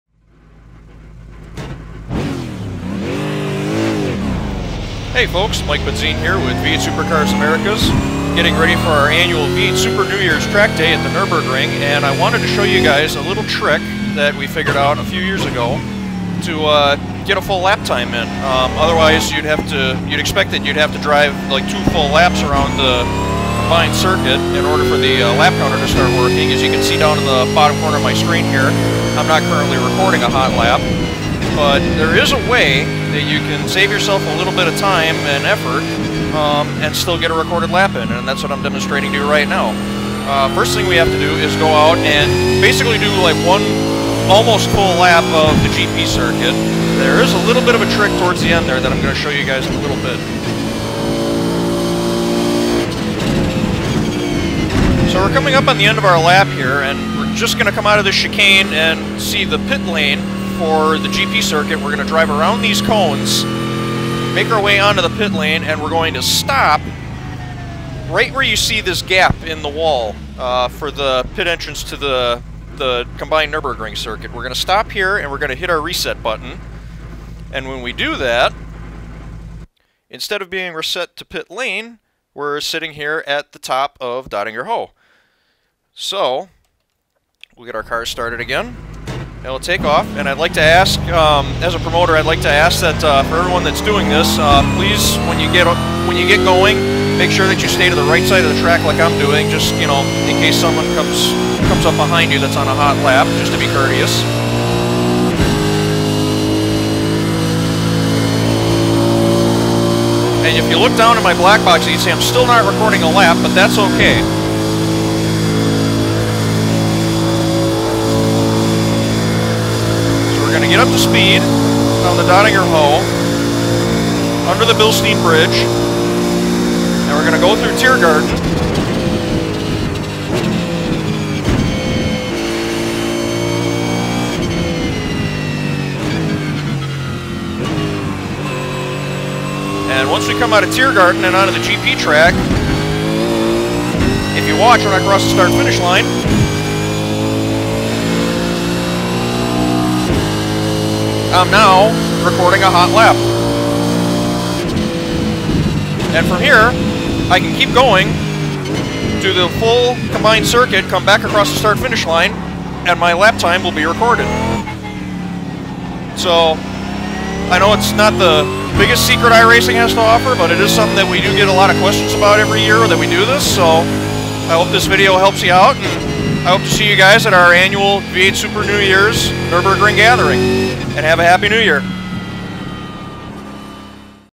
How to set a timed lap at the Combined 'Ring
Something we always get asked at our V8 Supercars Americas "V8 Super New Year" Open Practice track party is how to get a recorded lap in? Turns out there's a little trick that allows you to start at the top of Dottinger Hohe (the last long straight on the Nordschleife before Tiergarten and the GP track) and have the lap timer start just as you cross the finish line